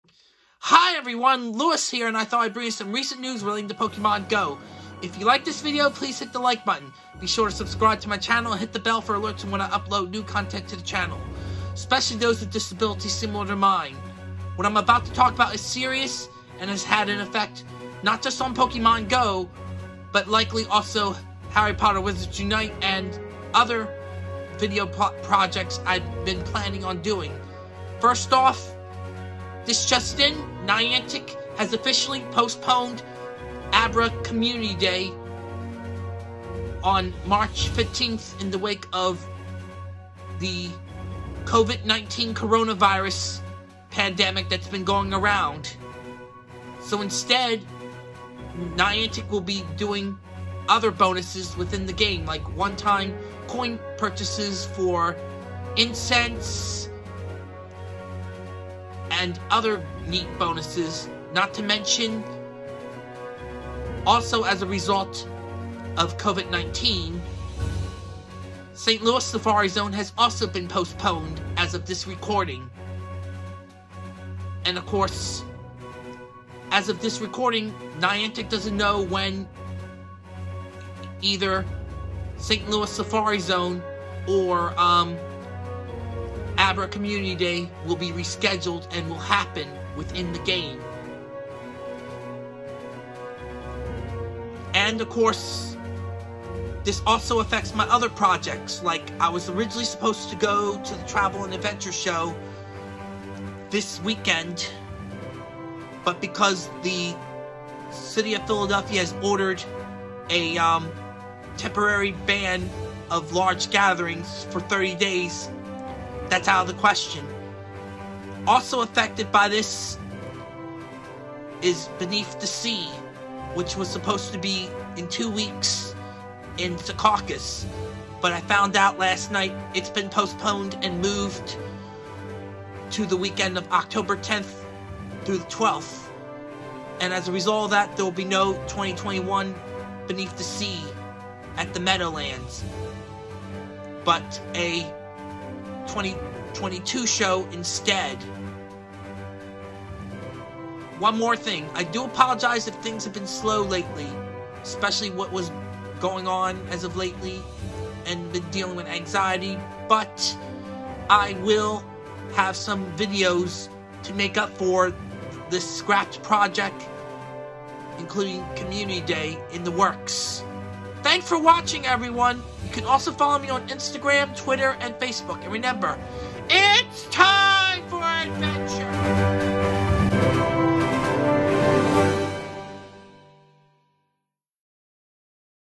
Pokémon Go News Episode 143 (Cancellations & Postponements due to the Covid-19 outbreak)
In this video I talk about how the Covid-19 Coronavirus which has been in the news has postponed events within Pokémon Go from St. Louis Safari Zone to Abraham Community Day, Harry Potter Wizards Unite with its community day, and my next documentary about the Travel & Adventure Show which I will go back to next year instead, & Beneath the Sea which has been postponed until the weekend of October 10th-12th.